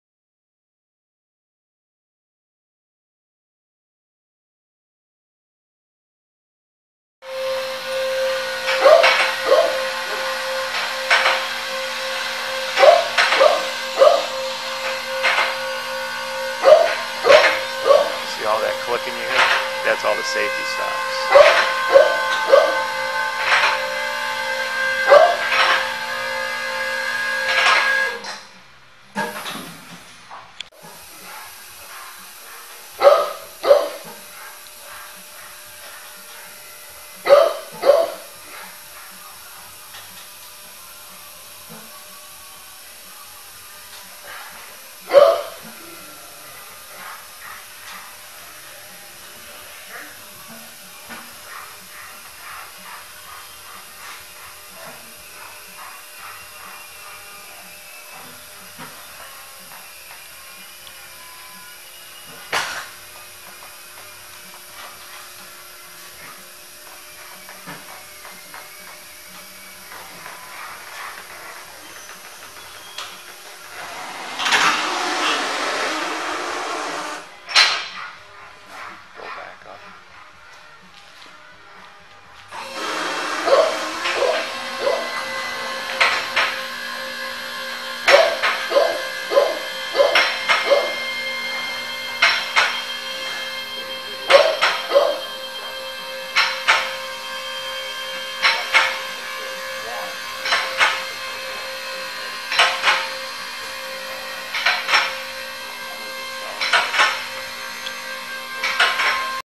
2003 Stertil Koni portable truck lift for sale | sold at auction April 8, 2015
2003 Stertil Koni portable truck lift
Model ST-1072
(4) lifts with one master lift
5&#039;H max lift to bottom of wheel lift
16,000 lbs max lift capacity
Power cord
(4) connector cords
Serial TH99428

It was located in Peotone, IL, and we sold it to the highest bidder for $10,450.